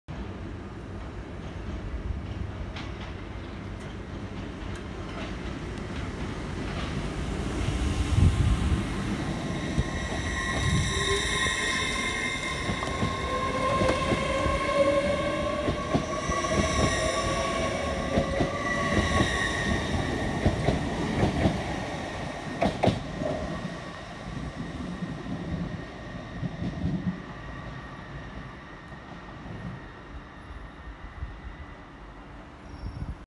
HullTrains 802 305 departs Doncaster Station with a London Kings Cross to Hull service
HullTrains class 802 305 seen departing Doncaster Railway Station working the 09:48 London Kings Cross to Hull service and will be calling at: Selby, Howden, Brough & Hull on Wednesday 19 of January 2022